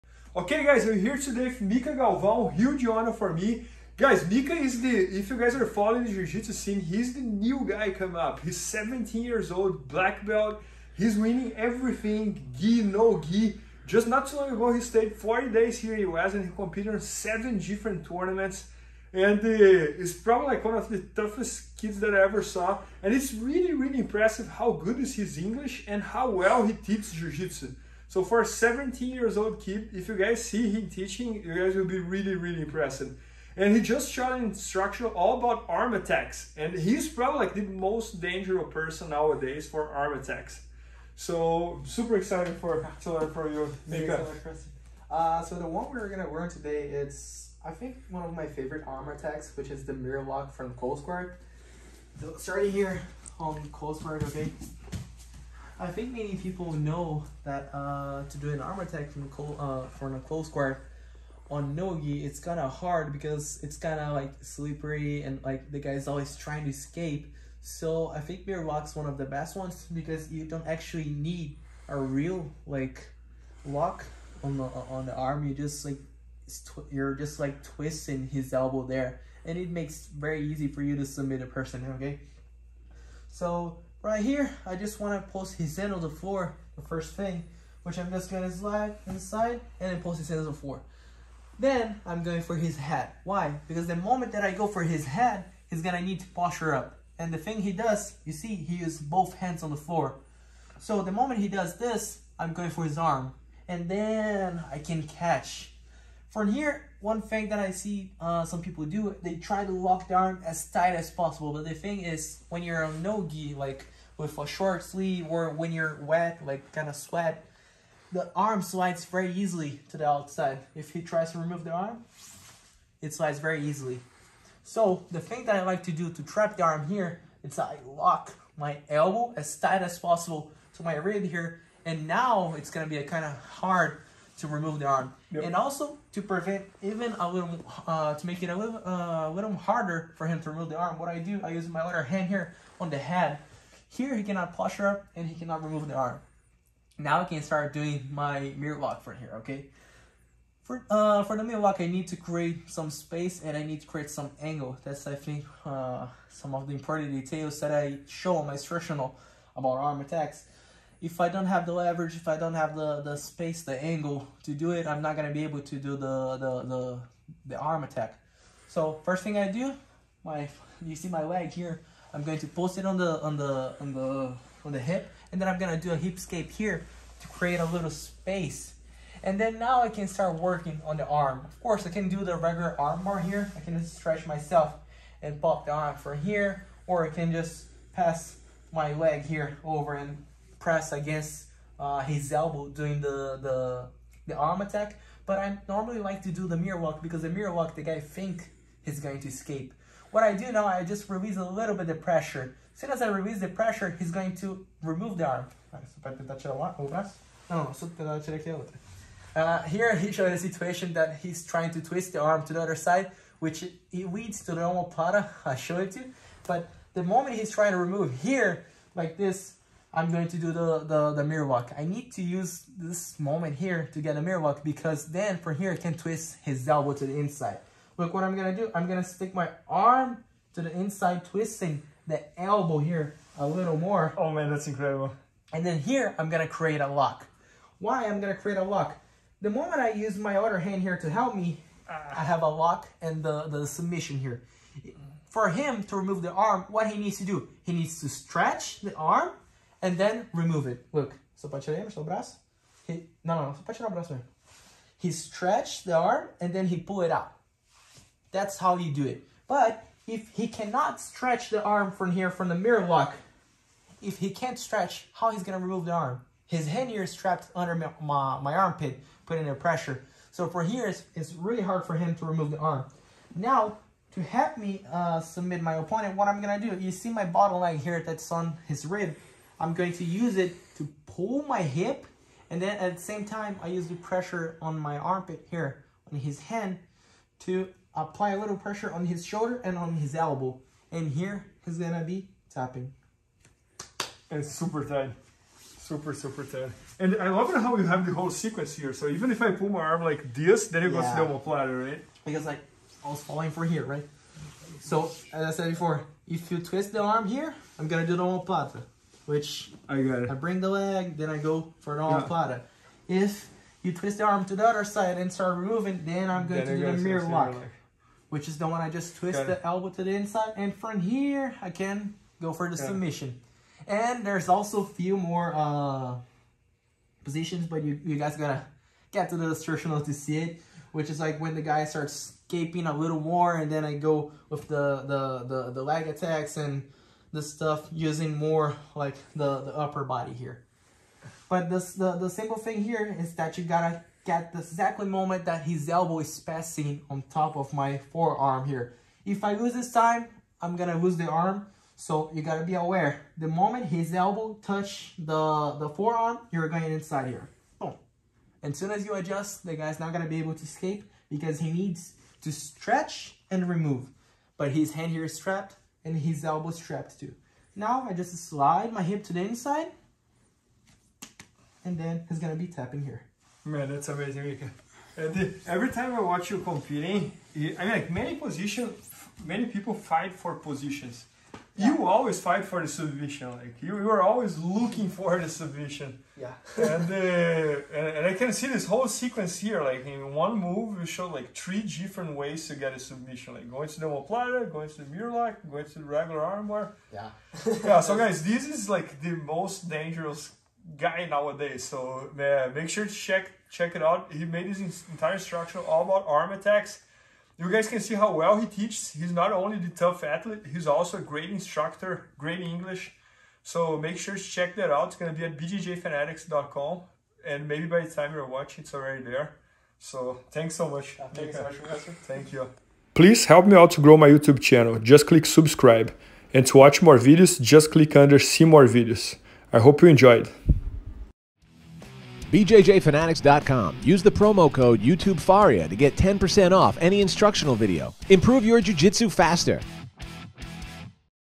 The Mir Lock (Jiu Jitsu Arm Attack) by Mica Galvao
Check out the submission that Mica Galvao is using in most of his matches. The Mir Lock.